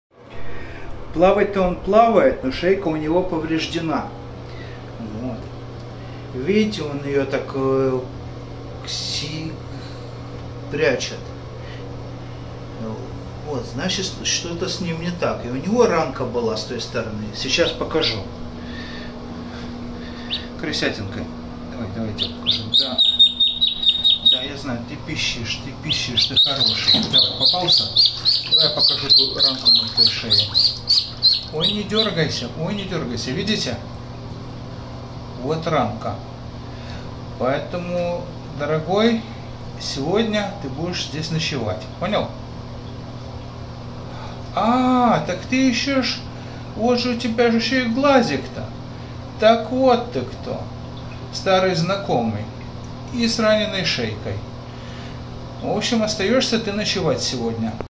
One eye blind Muscovy duckling with a peck wound on his neck
Очередное спасение слепого на один глаз утёнка